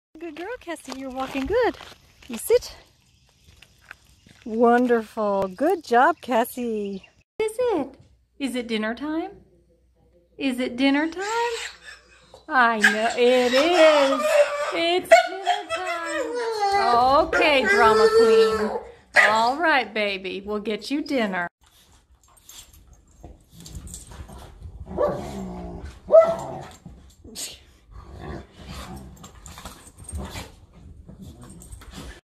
Meet Cassie Dee! She is available for adoption through Lending Paws a Hand!
Cassie Dee is a sweet pint-size pit mix that is looking for a forever home to call her own! She does great on a leash and also loves playing with dog friends! If interested in adopting Cassie, an application can be filled out